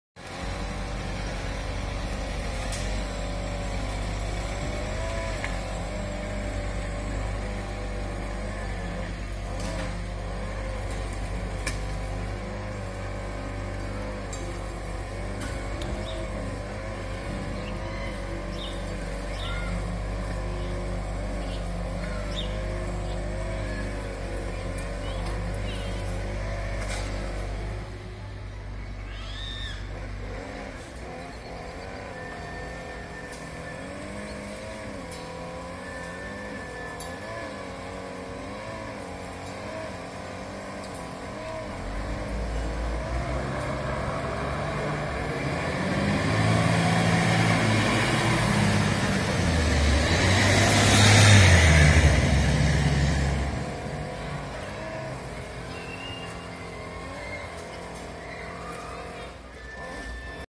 1 MINUT A TIVISSA 28 4 2022
1 Minut a Tivissa. Mentre les ampolles cauen del contenidor al camió, al camp de futbol del Freginal hi ha moviment. 11:12 h del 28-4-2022